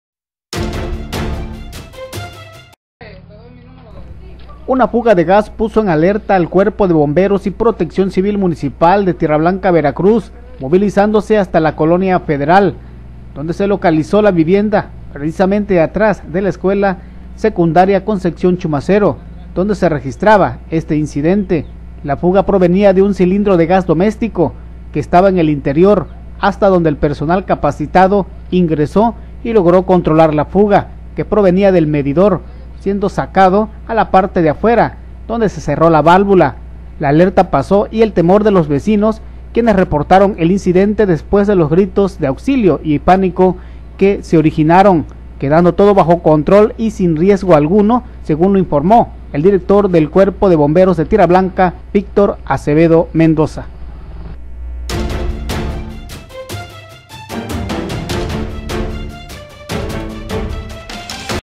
BOMBEROS COMBATEN FUGA
Una fuga de gas moviliza a bomberos